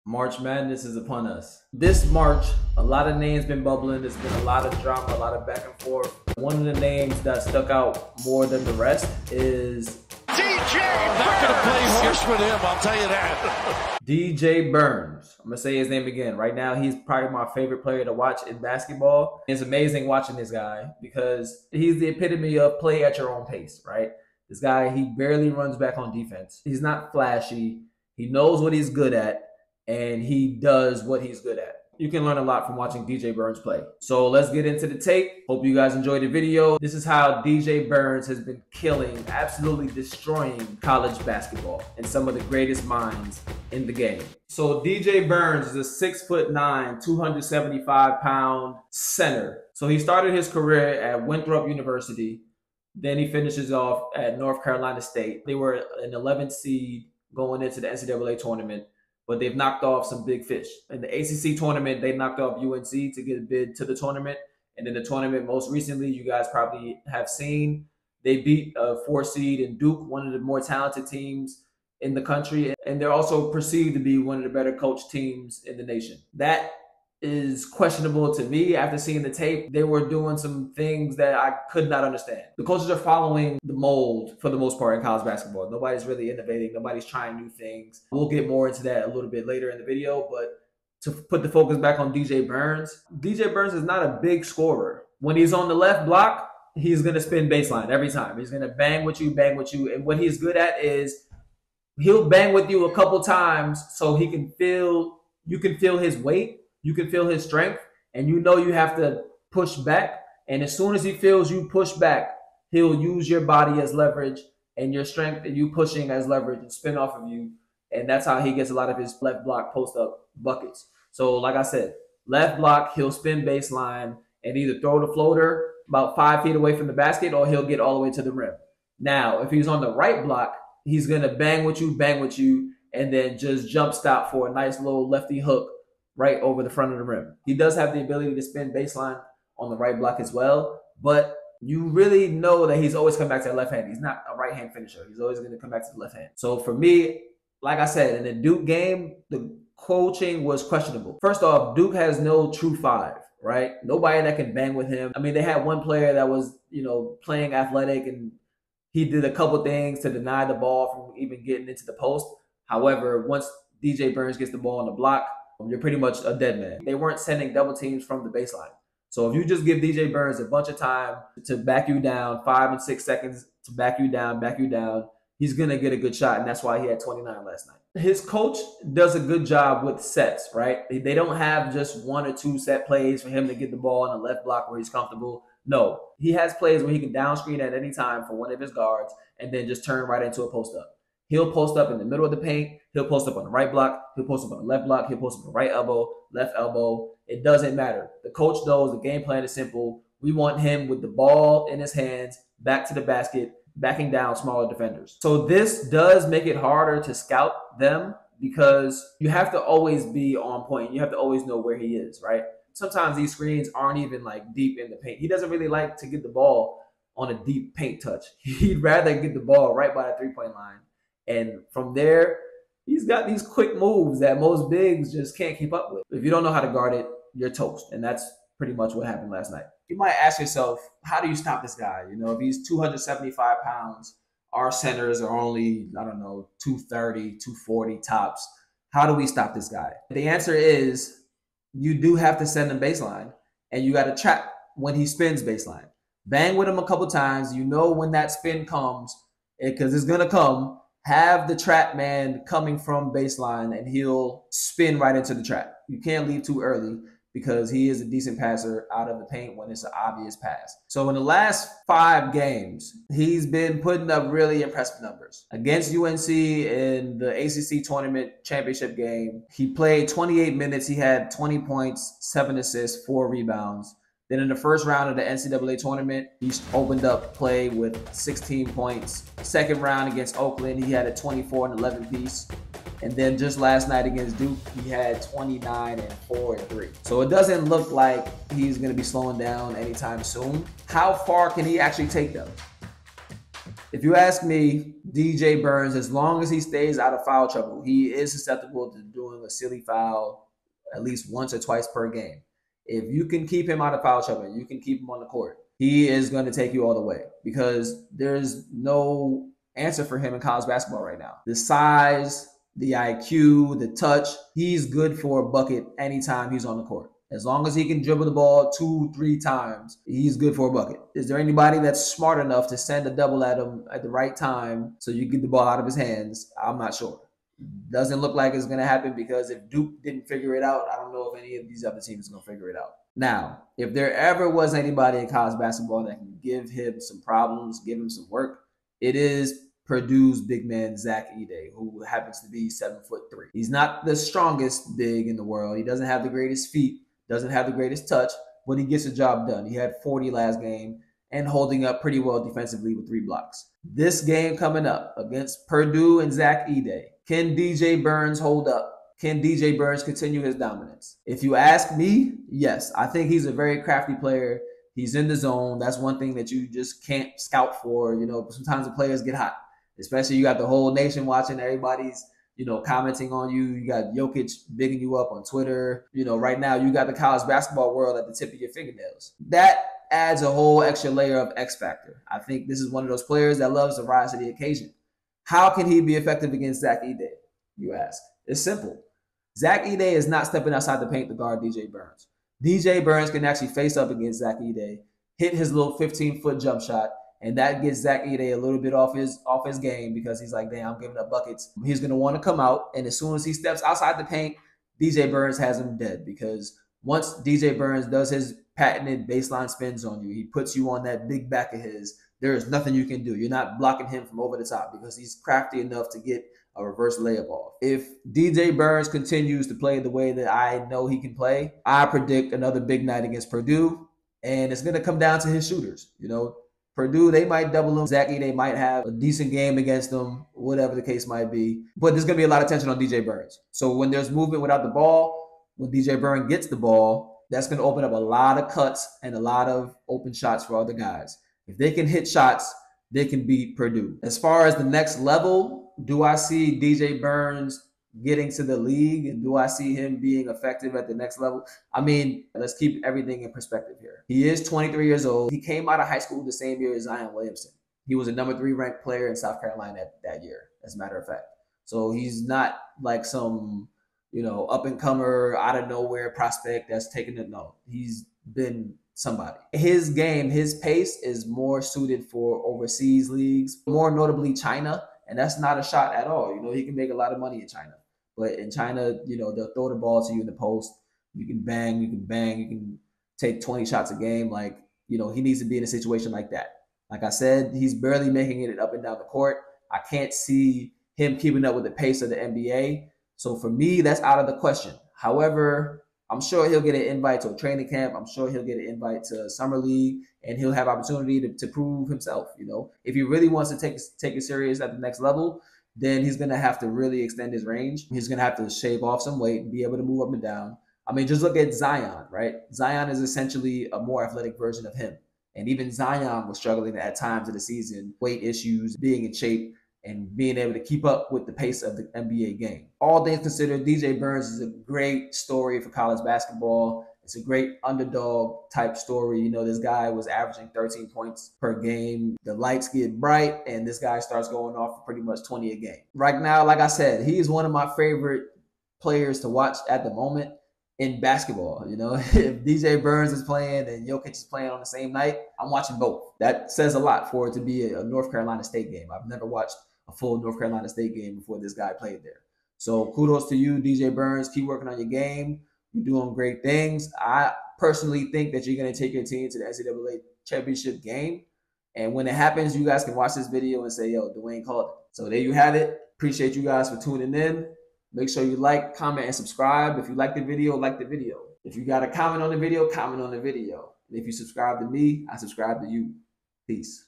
DJ Burns is March Madness CHEAT CODE!! Breakdown of his Dominance Vs. Duke
Dj Burns came into Match Madness on a mission! Here we take a good look at his game and answer questions about his potential NBA future.